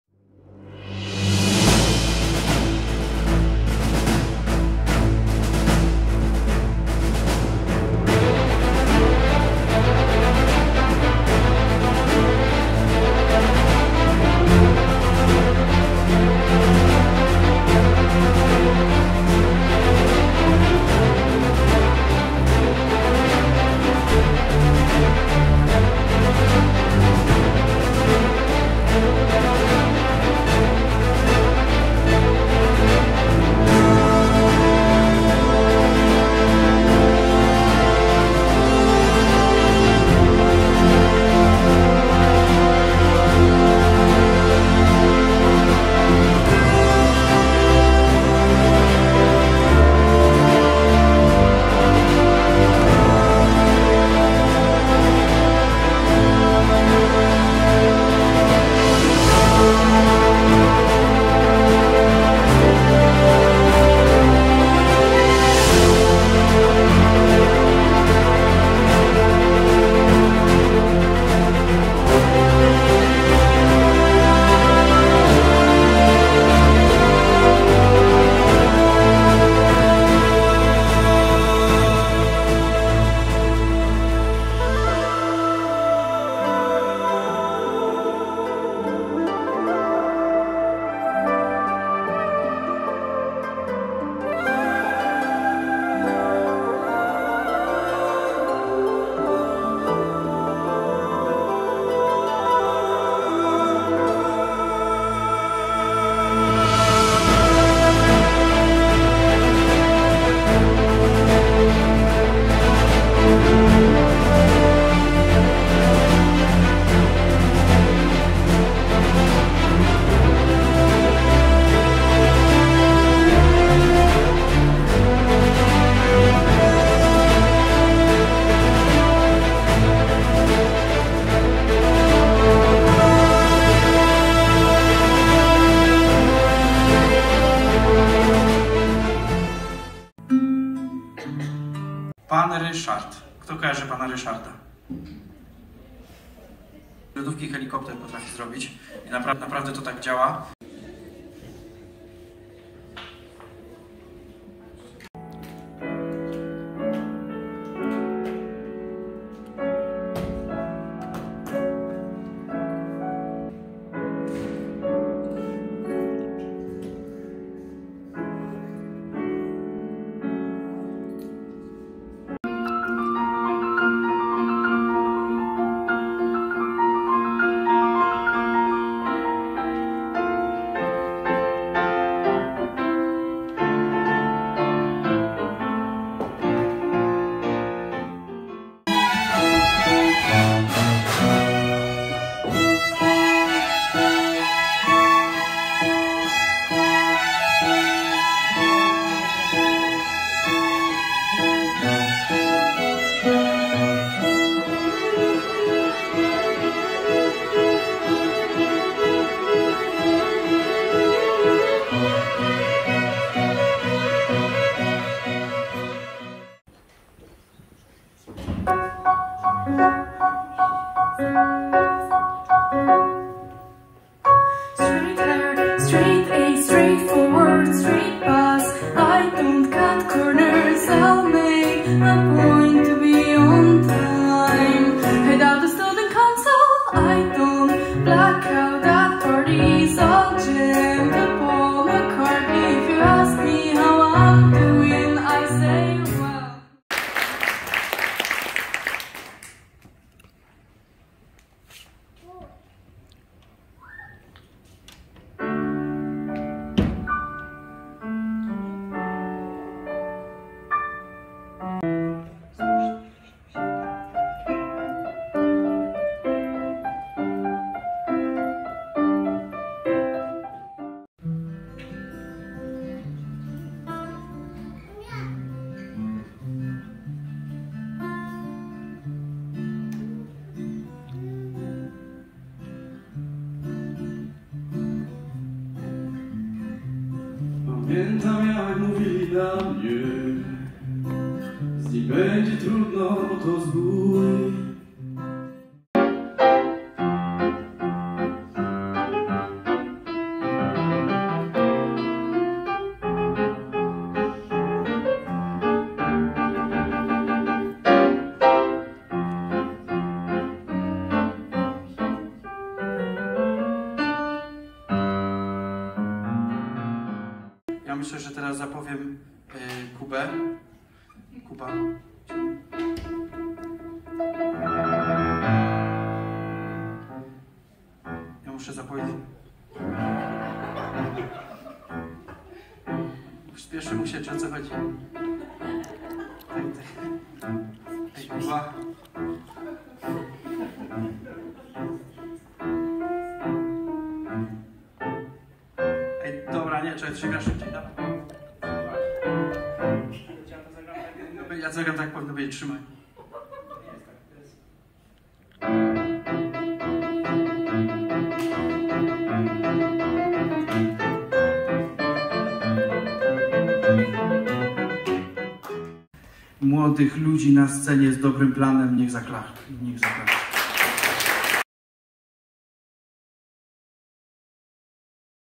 Tak było 16 czerwca w Żorach!
Koncert uczniów Stowarzyszenia Artystycznego "Żory"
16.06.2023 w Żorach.
fot. Joanna Krzyżanek
montaż: Jordan Krzyżanek